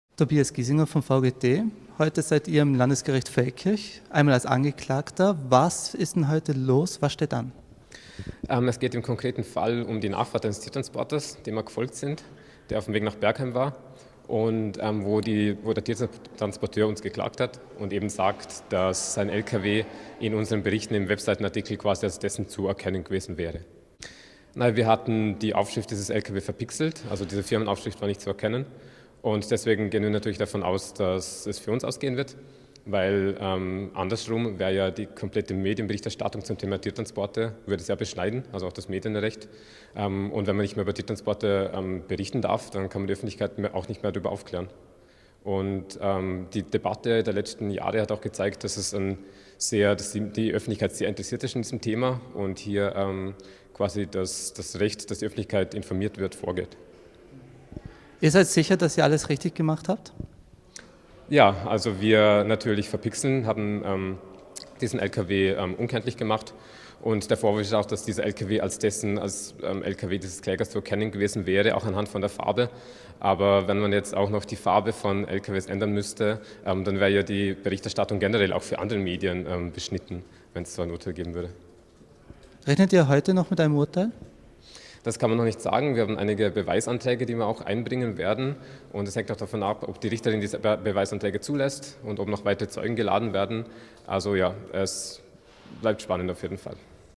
VGT in Vorarlberg auf Anklagebank
Ein Spediteur aus Tirol klagt den Verein gegen Tierfabriken vor dem Landesgericht Feldkirch. Seine Lkw sei in den Berichten des VGT über illegale Kälbertransporte erkennbar gewesen.